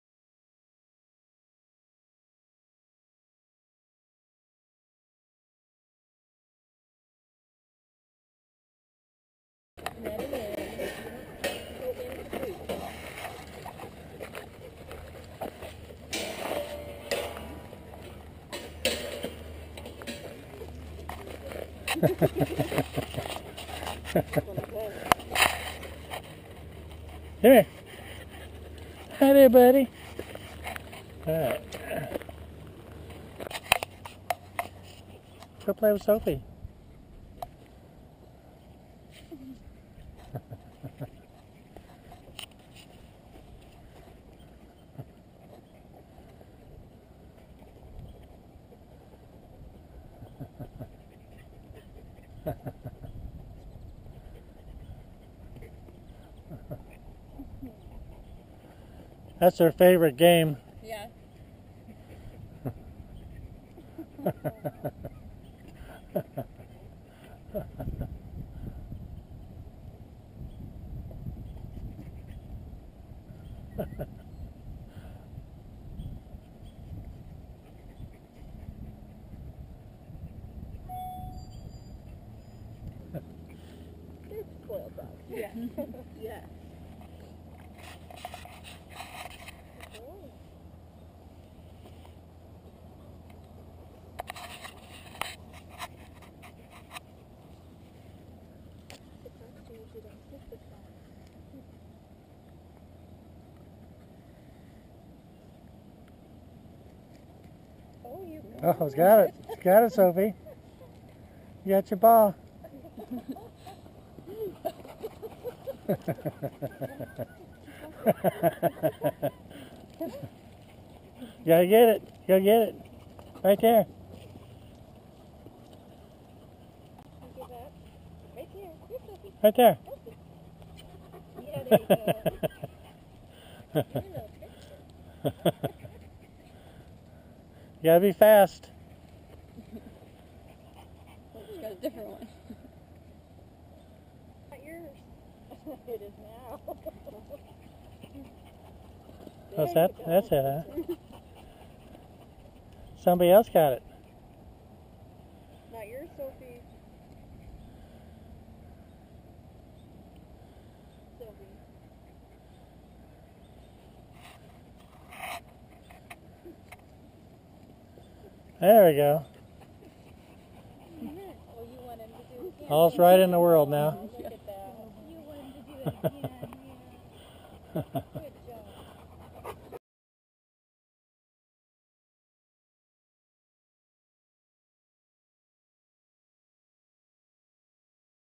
Sophie the Chocolate Lab Playing at the Dog park
Sophie, the Chocolate Lab, playing tennis ball keep away with Kiki.